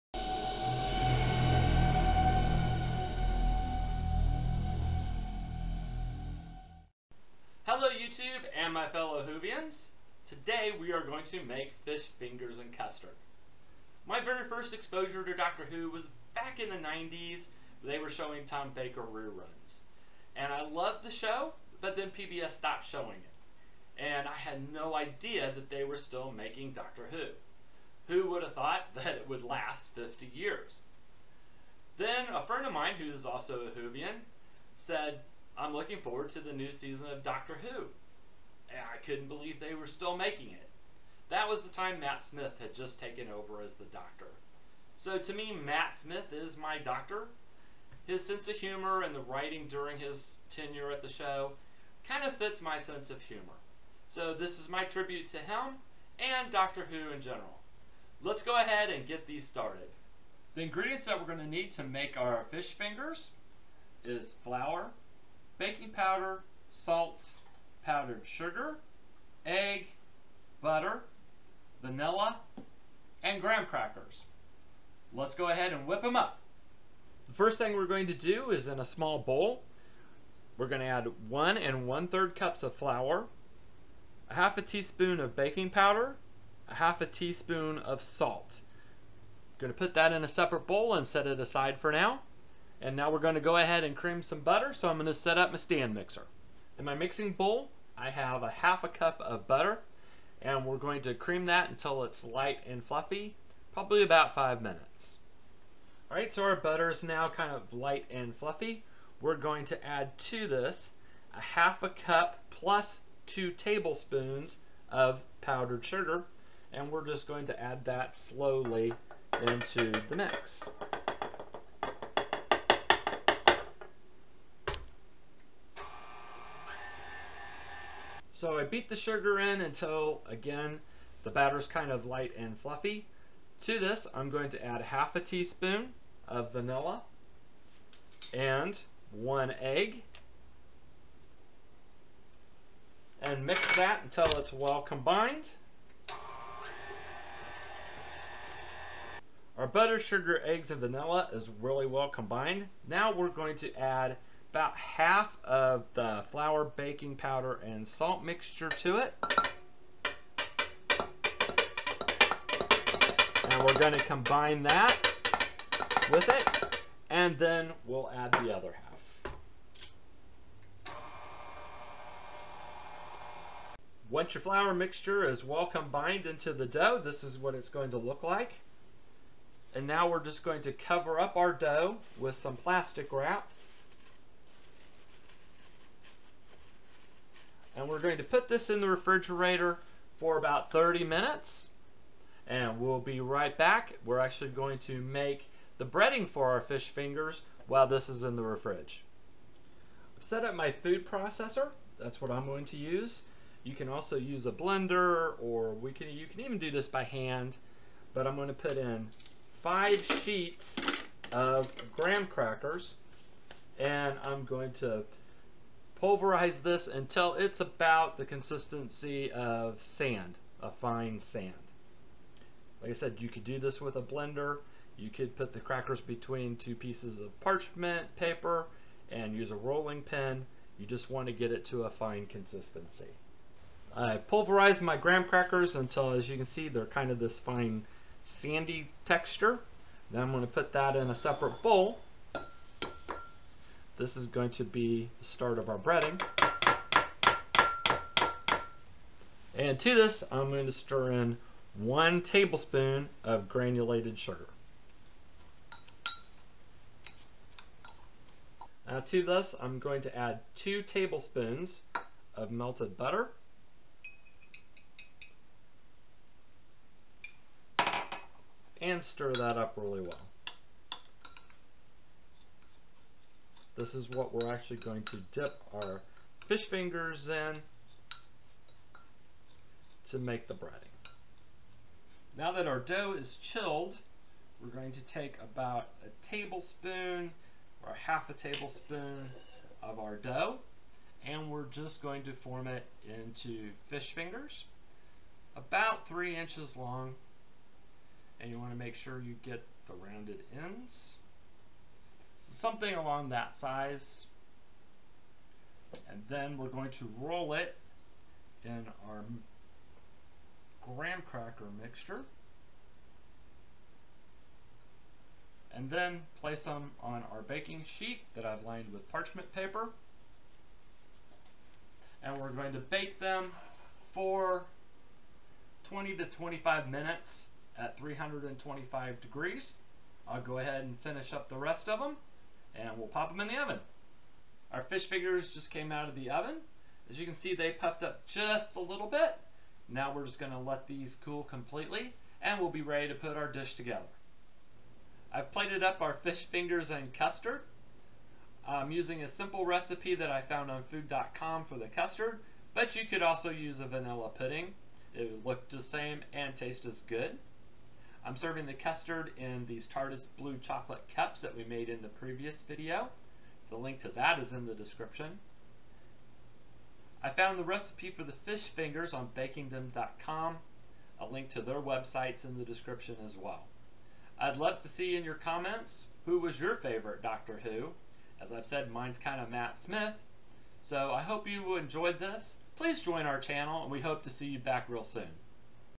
Doctor Who's Fish Fingers and Custard
Delicious butter cookies that  look like  fish  fingers (fish sticks). Served with a vanilla custard in  our TARDIS Blue Chocolate cups.  Great dessert for your next  Whovian get together. A celebration  of  the  new season of Doctor Who. Who  is your favorite Doctor? Leave answer in the comments.

Custard Recipe (Optional add a few drops of yellow food coloring.)

Fish fingers:
1/2 cup butter ( 1 stick)
1/2 cup plus 2 tbls powdered  sugar
1/2 tsp vanilla
1 egg
1 1/3  cups  flour
1/2 tsp baking powder
1/2 tsp salt

Breading:
5 sheets of graham crackers crushed to a fine sand consistency
1 tbls granulated  sugar
2 tbls  melted butter